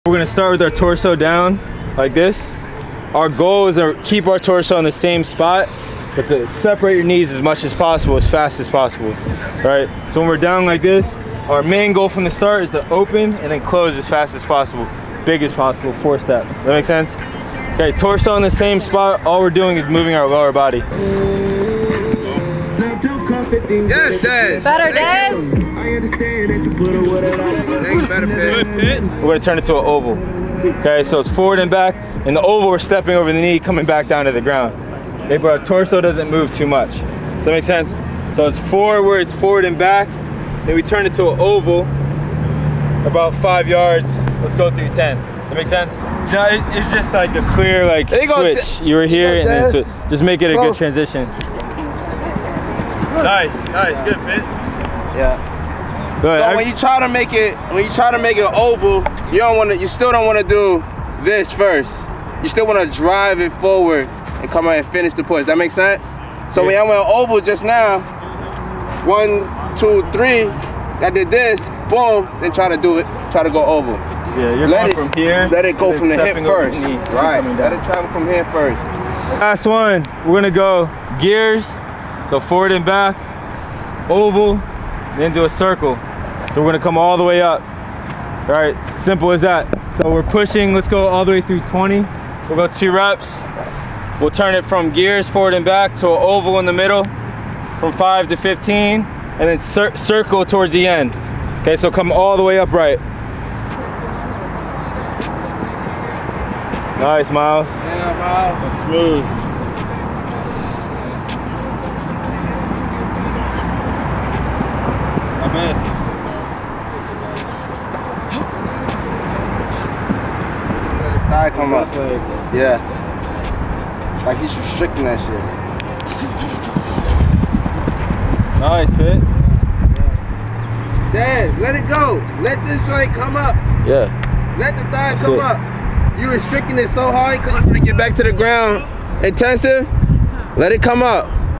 The 3 BEST Core & Leg Activation drills for any athlete
Acceleration is all about transitioning your torso and manipulating the action of the legs.

First three steps, get big and get small back attacking back behind your hips.

Transition by stepping down and turning the pattern into an oval shape then a circle. TAP IN WITH UNIVERSAL SPEED RATING